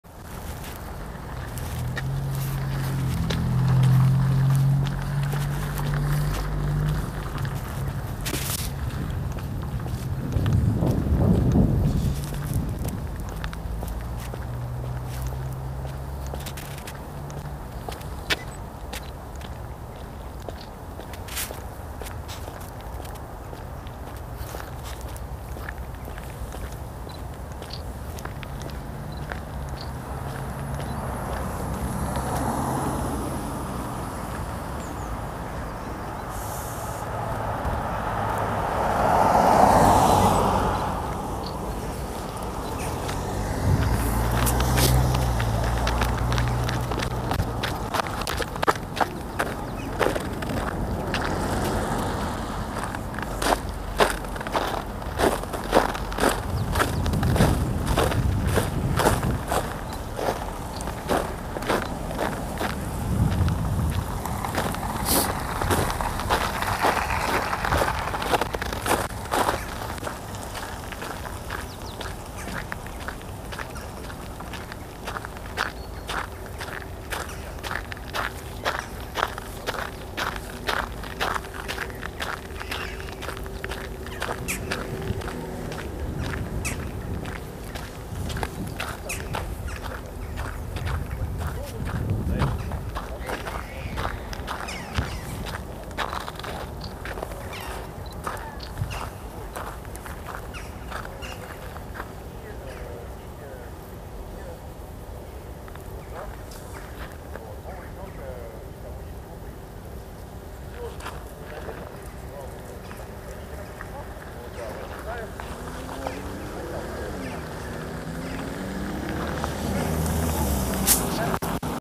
стройка рендома на Анненской улице, 6
20 этажей, видимо сдадут в 2023 году.

Гости канала! Перед просмотром видео прочитайте описание этого канала!